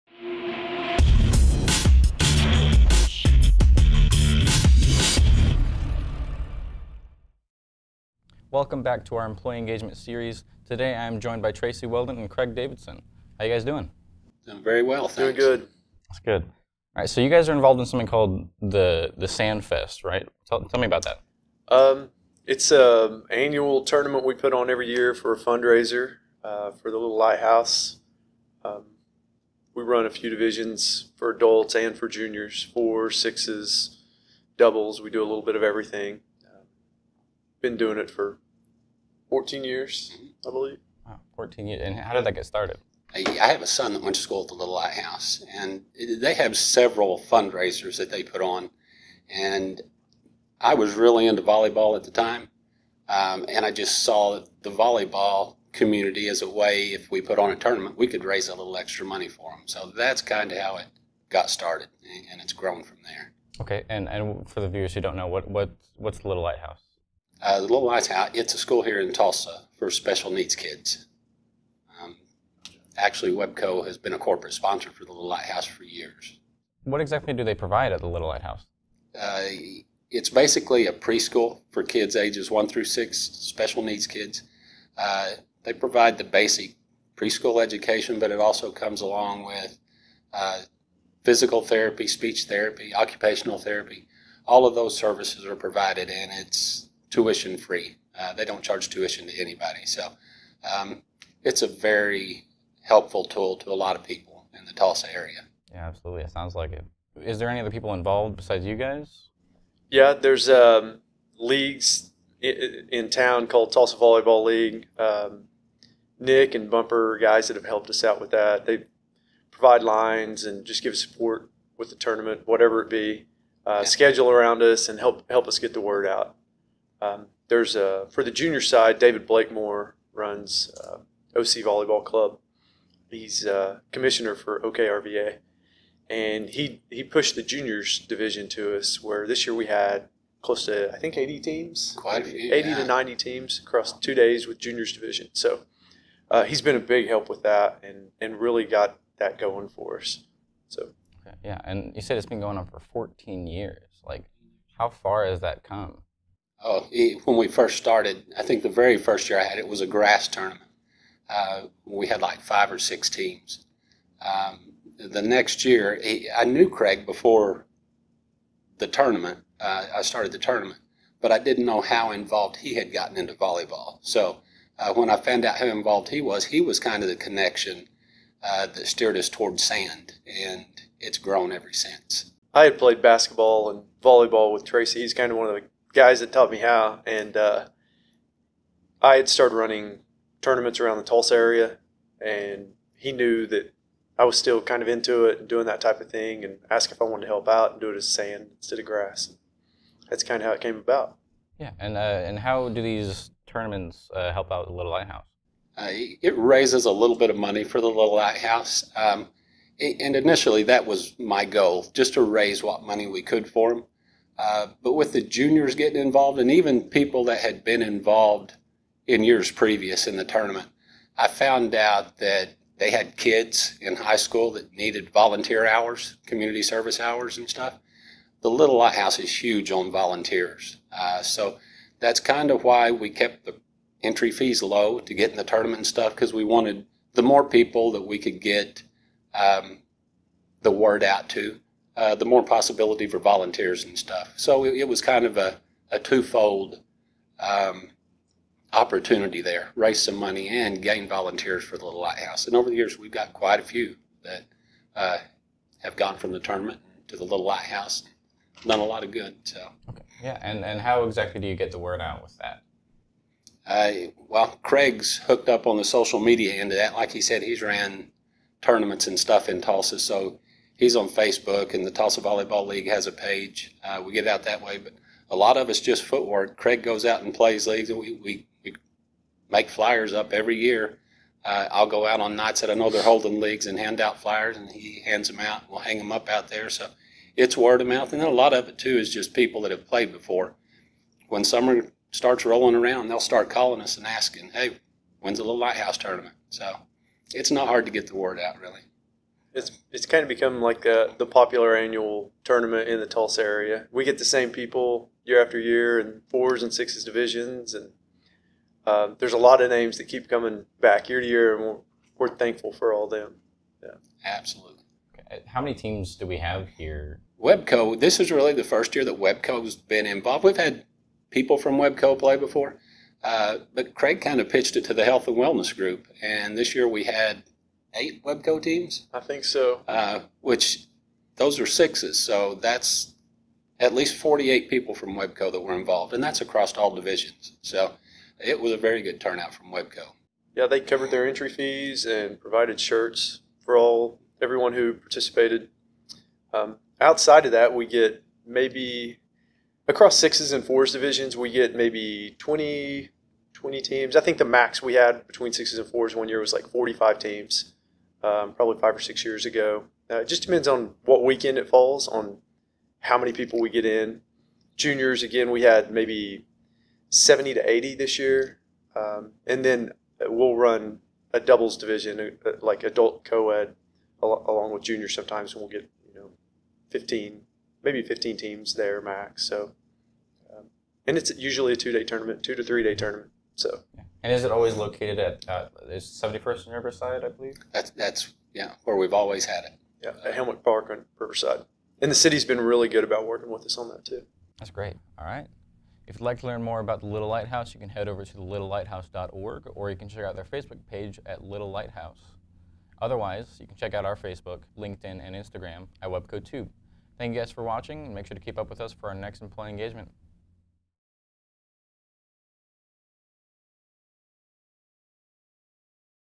Employee Engagement Series- Tracy Weldon and Craig Davidson
In today's video, we're introducing you to Tracy Weldon and Craig Davidson, the WebcoEmployees behind the annual Little Lighthouse Sandfest event. Their efforts help to raise money and gain volunteers for Little Lighthouse each year. Way to be 100% Engaged, even outside of work, Tracy and Craig! You are making a difference in the Tulsa community, and Webco appreciates you.